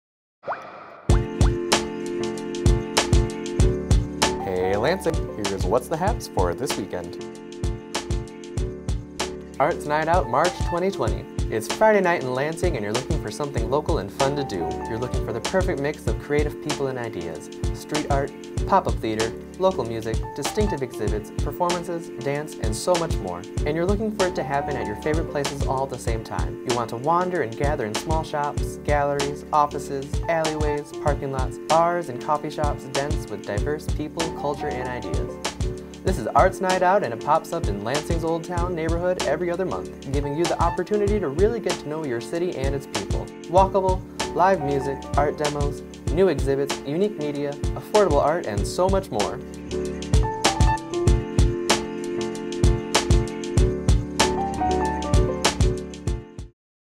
Arts Night Out Lansing Made
Fri | Mar 6 | 5:00 PM - 8:00 PM | FREE
Arts Council of Greater Lansing
1208 Turner St | Lansing | 48906

It’s Friday night in Lansing, and you’re looking for something local and fun to do. You’re looking for the perfect mix of creative people and ideas -- street art, pop-up theatre, local music, distinctive exhibits, performances, dance and so much more – and you’re looking for it to happen at your favorite places all at the same time. You want to wander and gather in small shops, galleries, offices, alleyways, parking lots, bars and coffee shops, dense with diverse people, culture and ideas.
This is Arts Night Out and it pops up in Lansing’s Old Town neighborhood every other month, giving you the opportunity to really get to know your city and its people. Walkable, Live music, Art demos, New exhibits, Unique media, Affordable art, and So much more!